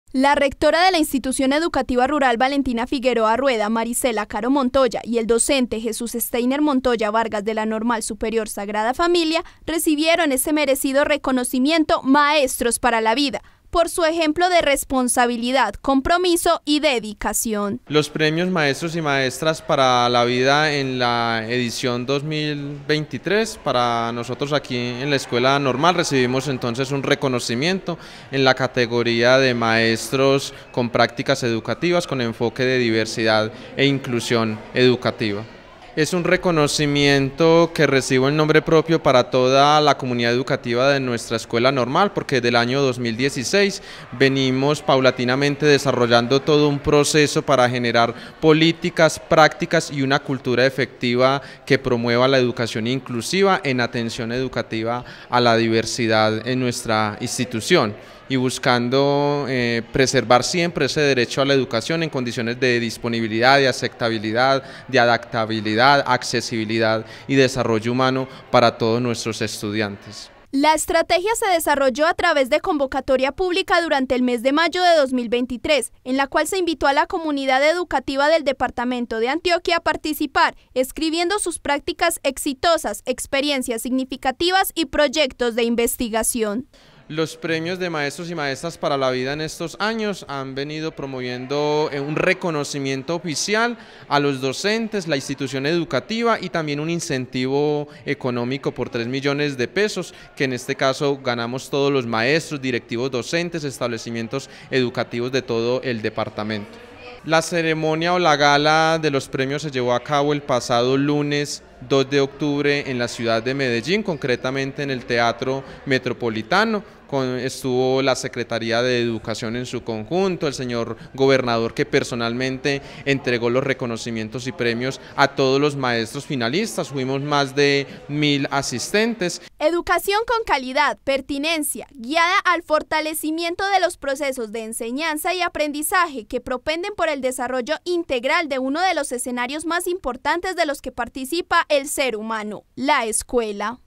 "Maestros para la vida" Exalta doncenes urraeños
Maestros, Directivos Docentes y Estudiantes de las Instituciones Educativas de los municipios no certificados del Departamento, participaron de la ceremonia de premiación y exaltación del Premio Maestros y Maestras para la Vida 2023. Dos docentes de Urrao recibieron reconocimiento